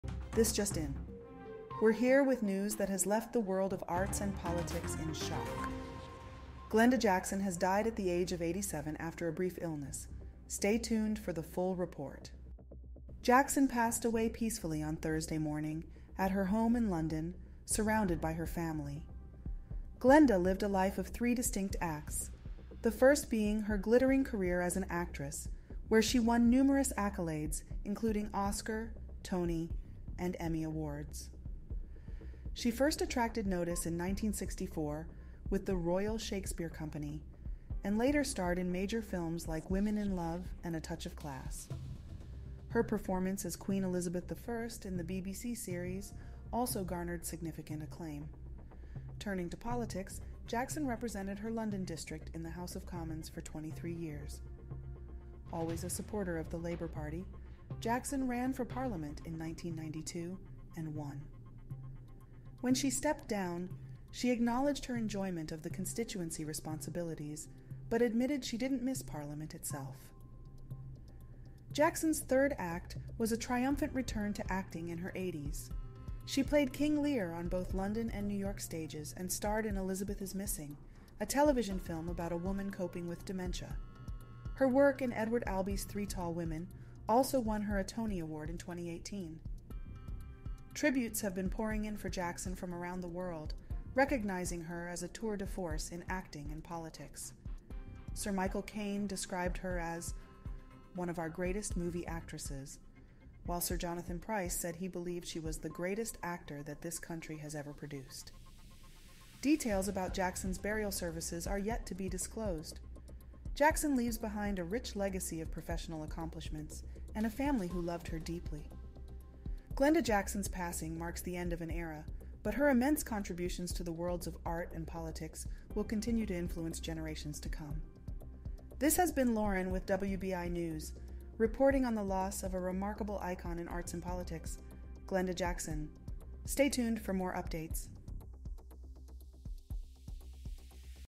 Labour MP Glenda Jackson dies at 87 [Breaking]
In this special WBI News report, we honor the life and legacy of Glenda Jackson, one of the world's greatest actresses and a respected former member of Parliament, who has sadly passed away at the age of 87. Jackson's career spanned over several decades, transitioning from the stages of the Royal Shakespeare Company to the big screen, winning numerous prestigious awards along the way. She later served her London district in the House of Commons for 23 years, and upon stepping down, made a triumphant return to acting in her 80s.

Throughout this video, we delve into Jackson's remarkable life, her outstanding career in both arts and politics, and the immense influence she leaves behind. From tributes pouring in from around the world to her pivotal roles and political endeavors, join us as we remember the extraordinary life of Glenda Jackson.

Leave a comment to share your favorite Glenda Jackson performance or how she may have influenced you.